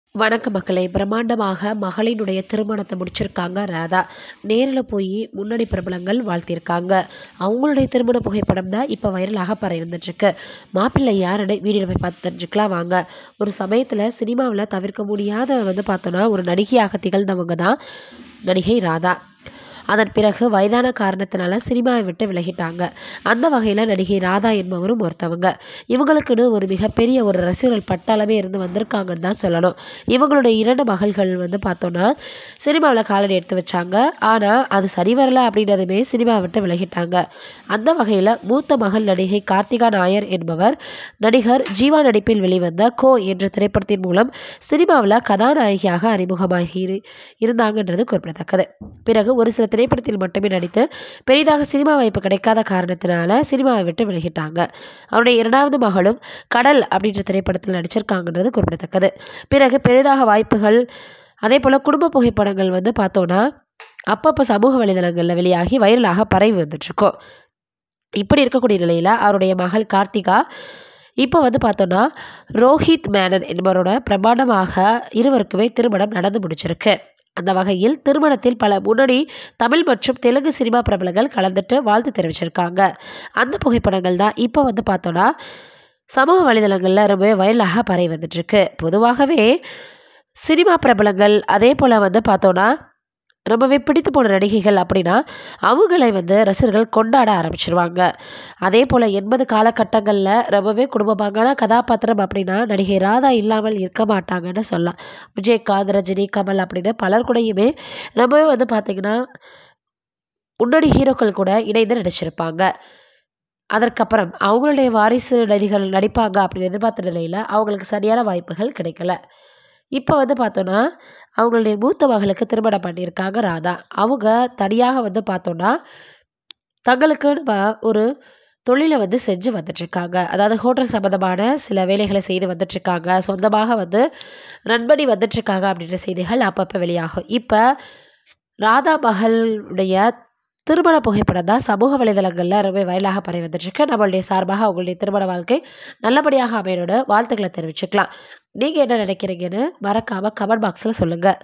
பிரமாண்டமாக மகளின் திருமணத்தை முடித்தராதா?நேரில் சென்று வாழ்த்திய பிரபலங்கள்..!!வைரலாகும் புகைப்படம்
பிரமாண்டமாக மகளின் திருமணத்தை முடித்த ராதா.? நேரில் சென்று வாழ்த்திய முன்னணி பிரபலங்கள்..!! வைரலாகும் திருமண புகைப்படம்..!!